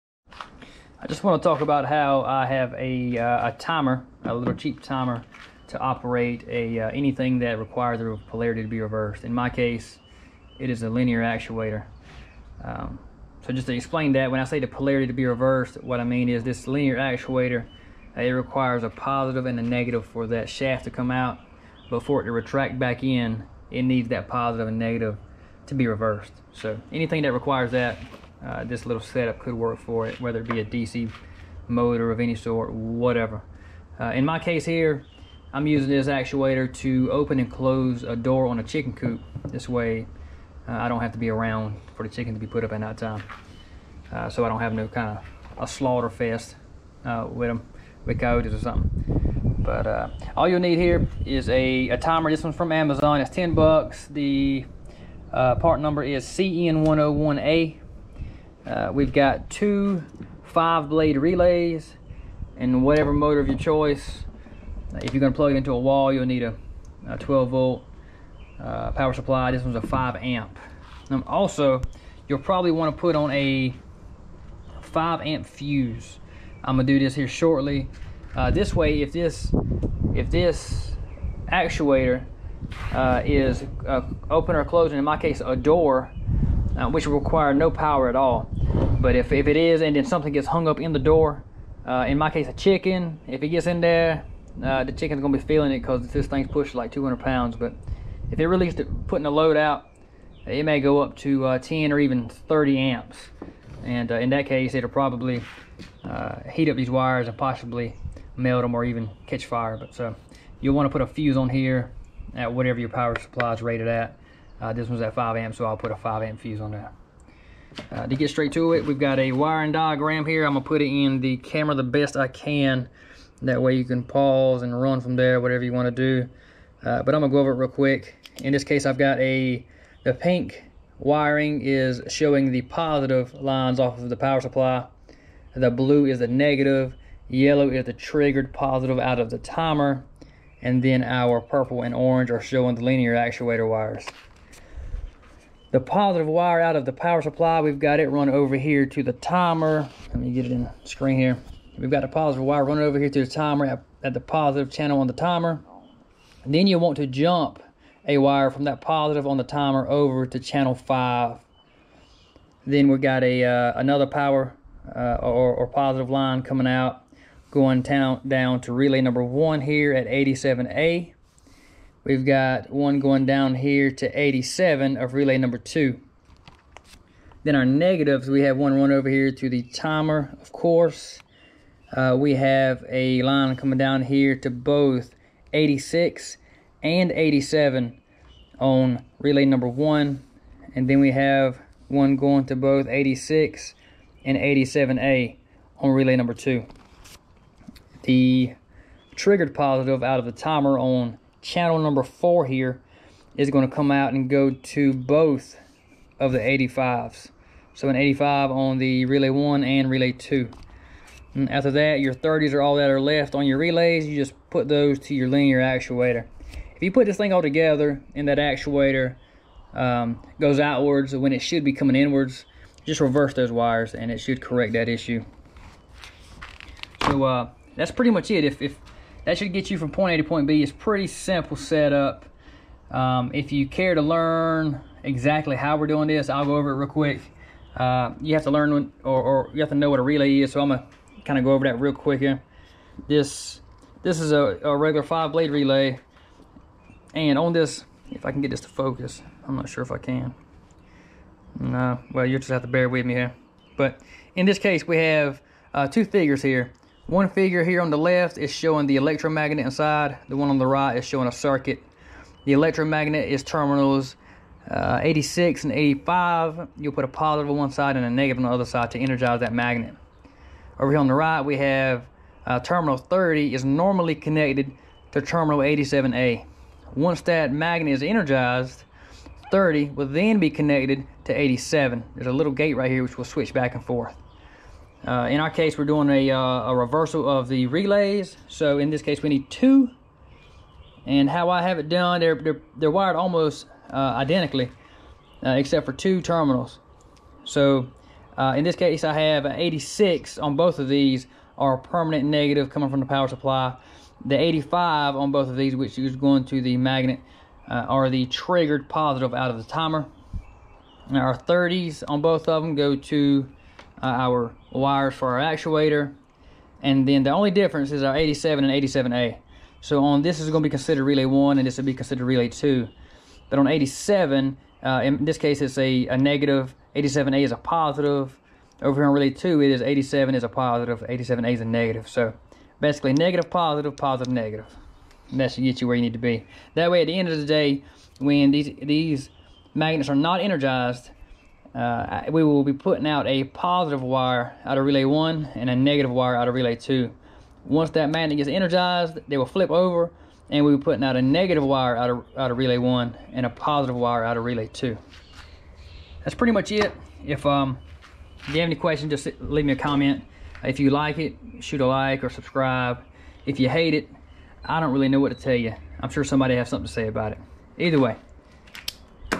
How to: Linear actuator, timer, 2 relays. Automatic chicken door!
Wiring diagram and quick run-through on how to reverse polarity/wire a timer to a linear actuator using 2 relays. Automatic chicken coop door, etc..

CN101A timer

5 amp 12v power supply

2x 5 pin relays

Linear actuator

5 amp fuse